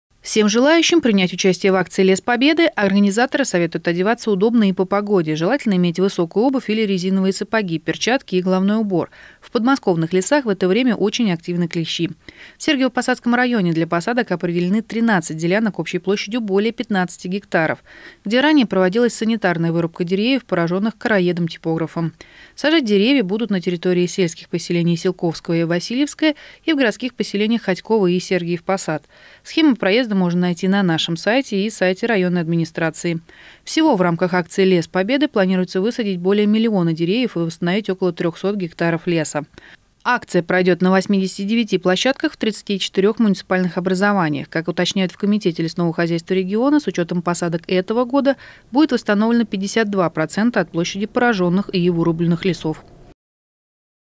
В эту субботу в Подмосковье пройдёт акция «Лес Победы»
Её центральной площадкой станет квартал дачного участкового лесничества вблизи Раменского, сообщает пресс-служба комитета лесного хозяйства Московской области. Всего планируется высадить более миллиона деревьев в 34-х муниципальных образованиях. В Сергиево-Посадском районе сажать деревья будут на территории Селковского и Васильевского, а также в Хотьково и Сергиевом Посаде.